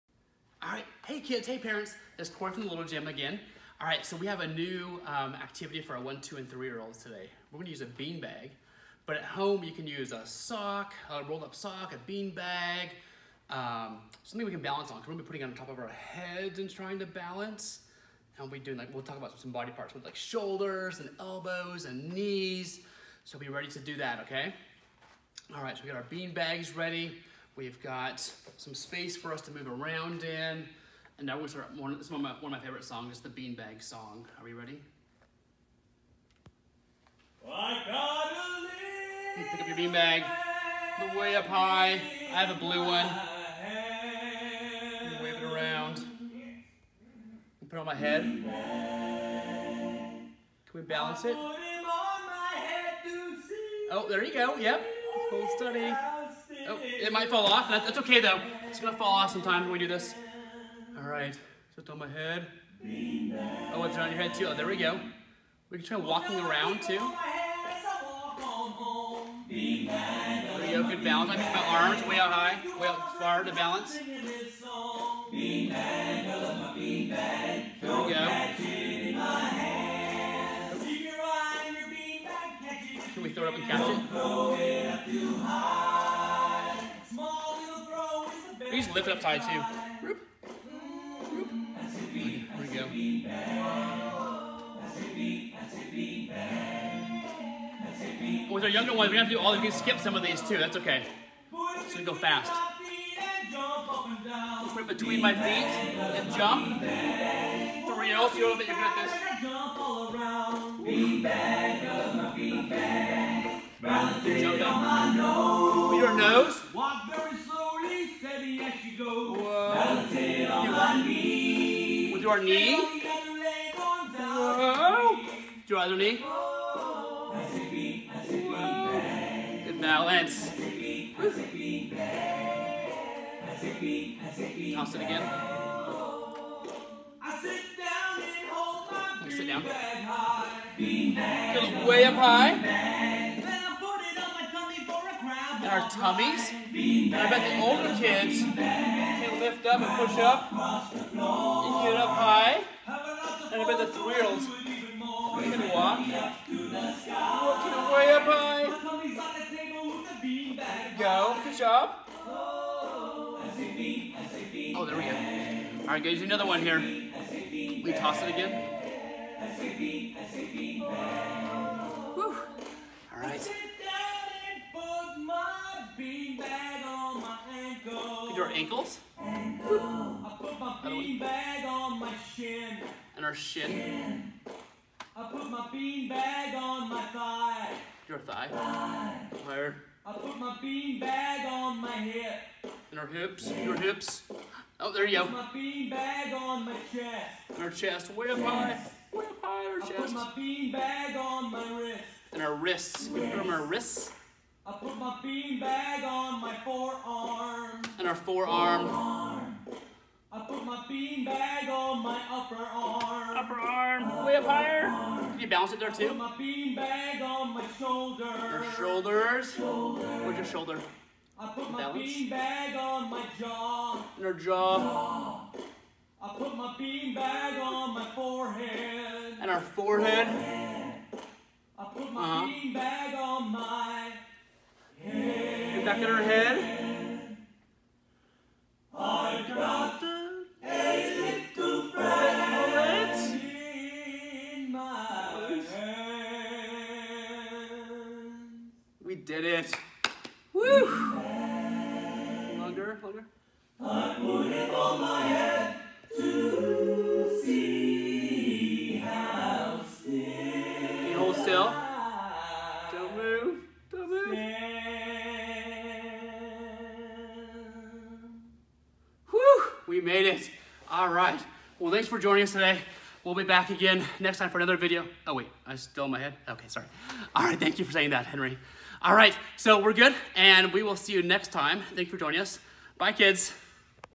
Balancing Bean Bags! [Parent Child/Pre-K 1, 2, and 3 Year Olds]
Here we use beanbags to work on our balance with our littlest ones. Mr Corey takes his interpretation of one of his favorite songs from class and turns it into fun for you and your 1, 2, and 3 year olds!

If you don’t have a beanbag at home (I mean who really has one at home?) you can use a rolled up sock or a folded washcloth or dish towel!